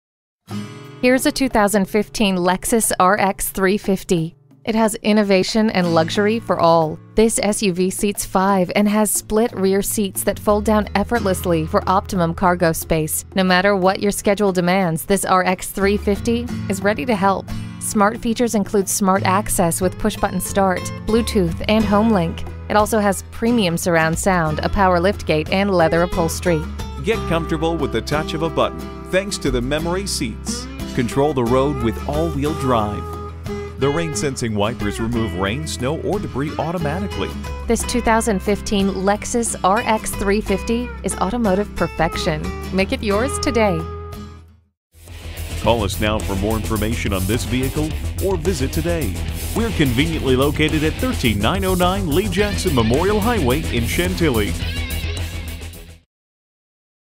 New 2015 Lexus RX 350 Chantilly VA Washington-DC, MD RXFC70351 - SOLD
Year: 2015
Make: Lexus
Model: RX 350
Trim: AWD 4dr
Engine: L 6 Cyl.
Color: Claret Mica
Interior: LC00
Stock #: RXFC70351

Address:
13909 Lee Jackson Memorial Hwy